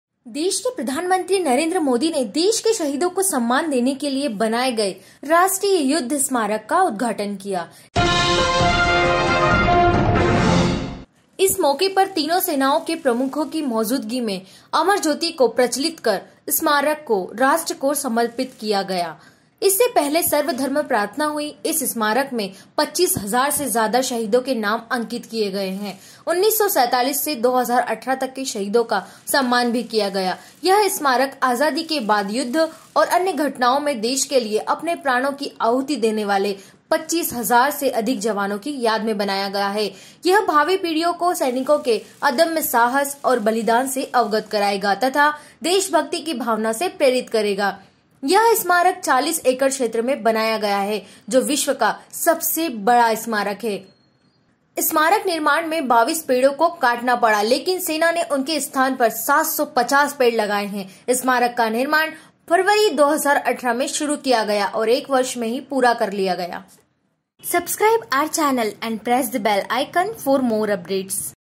National War Memorial: Narendra Modi Pays Tribute to MARTYRS - मोदी ने सौंपा शहीद स्मारक राष्ट्र को
For the first time in Memory of the Martyrs, such a great Monument.

Prime Minister Narendra Modi inaugurated the National War Memorial to honor the martyrs of the country. On this occasion, the memorial was dedicated to the nation by introducing Amar Jyoti in the presence of the heads of the three armies.

Earlier this prayer of all religions has been mentioned in the memorial, more than 25000 martyrs have been named, the martyrs of 1947 to 2018 were also honored, after the independence of India, in the war and other events, sacrificing their lives for the country 25 thousand Made in memory of more Jawans, this future generations will be untold courage of soldiers and will sacrifice the country and will inspire the spirit of devotion, this memorial has been built in 40 acres of area, the world's largest monument, 22 trees have been cut in monument construction but Army has installed 750 trees in their place. Started in 2018 and completed in 1 year only.